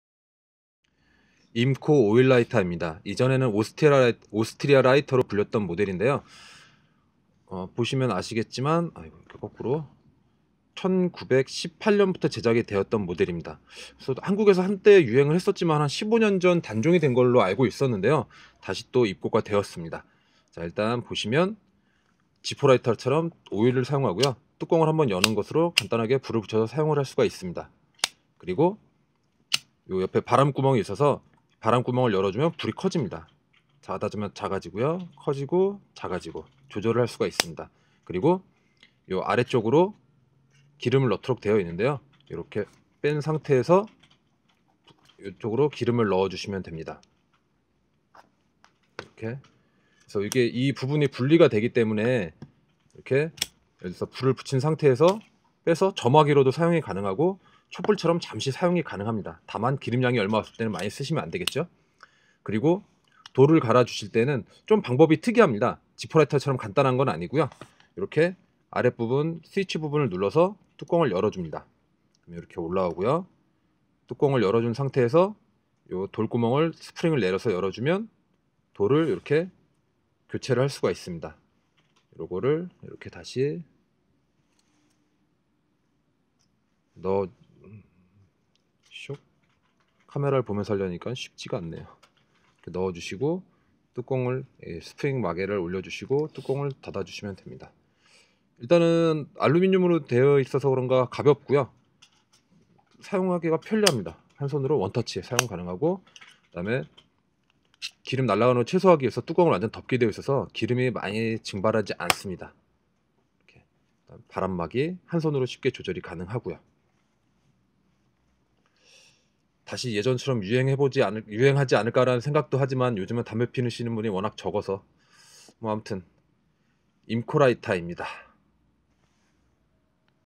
IMCO 임코 오일 라이터 : 오스트리아 라이터 [한강사]
아주 예전에는 오스트리아 라이터로 유명했던 임코 오일 라이터입니다.
오일을 사용하는 라이터로 매우 가볍고 사용이 편리한 장점이 있습니다. 다용도로도 사용이 가능합니다.
국내 에이젼트가 있었지만..
판매량이 많이 않은 탓에.. 없어지고 말았네요~
그래서 국내에서 구경을 하기 힘들어진 모델입니다.